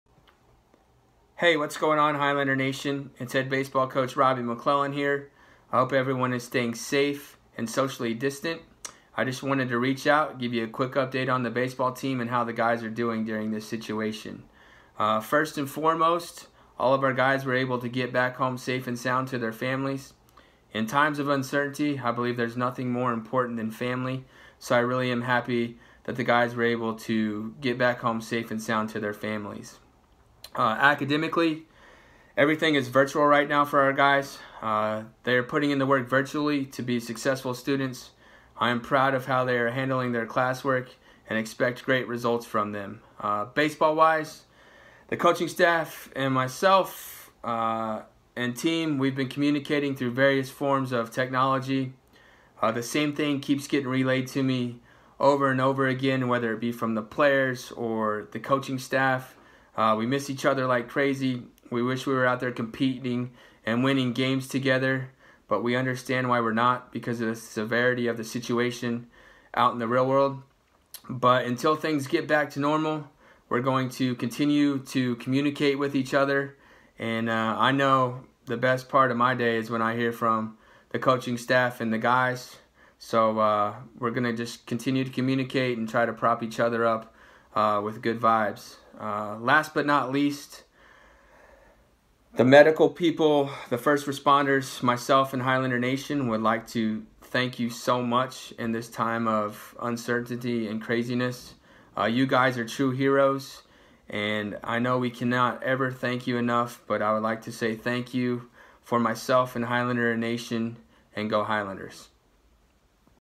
NJIT Kilted Club COVID-19 Update: Baseball
NJIT Baseball Head Coach Robbie McClellan gives a program update amid the COVID-19 pandemic.